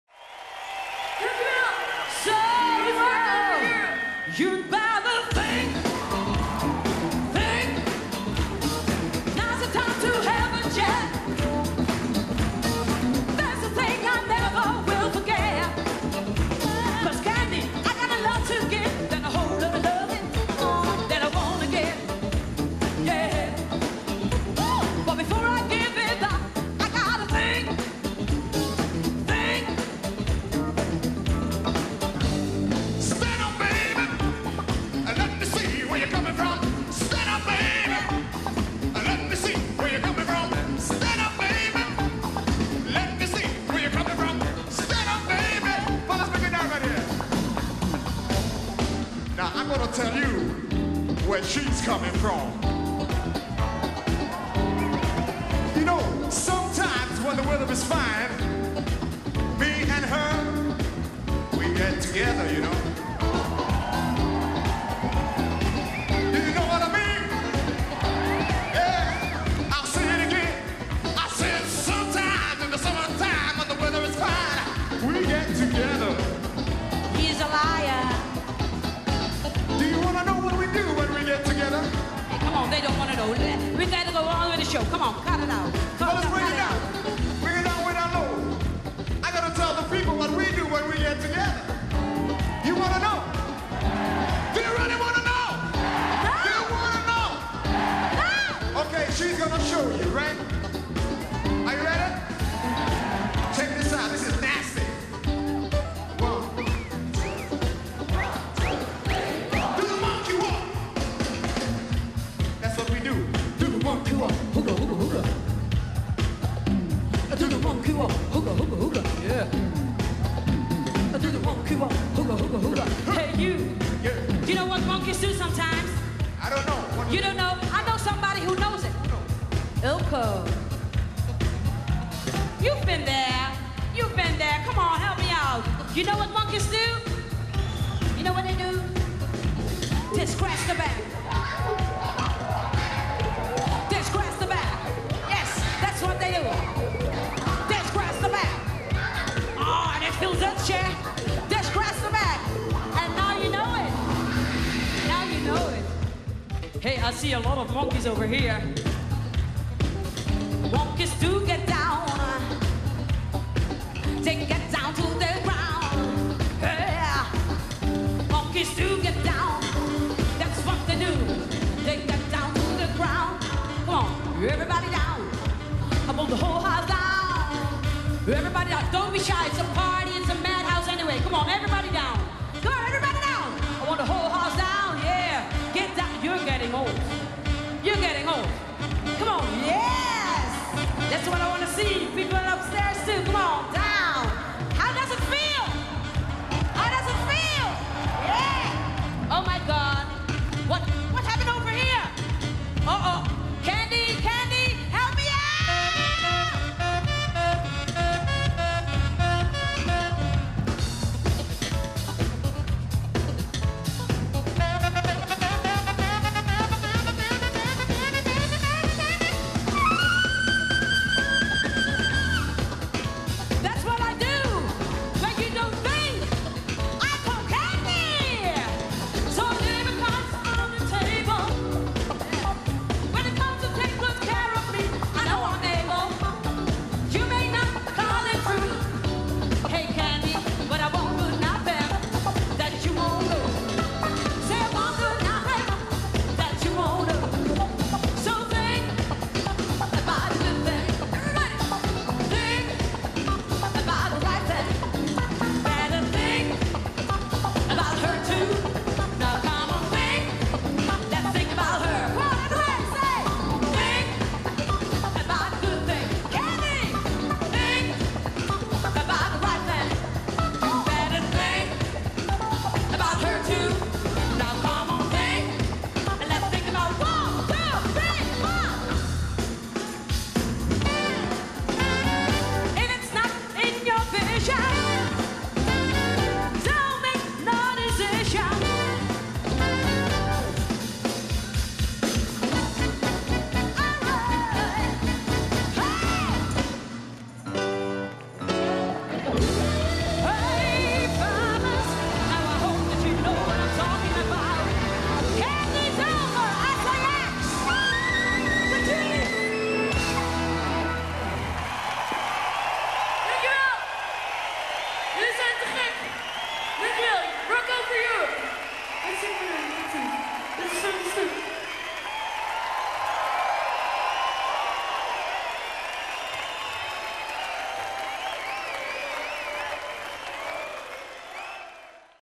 Candy Dulfer & Funky Stuff - James Brown Medley - Live 1991
A whole lot of entertainment has this wonderful footage of this succesfull combination of enthousiast and good musicians. Partytime with Candy Dulfer and Funky Stuff, enjoy, comment if you like. Besides Candy also with: Michel van Schie - bas, Hugh Kanza - vocals, Patricia Balrak - vocals, Anna Makaloi - vocals, Eveline Carels - drums, Ulco Bed - gitaar, Bobby van de Berg - keyboards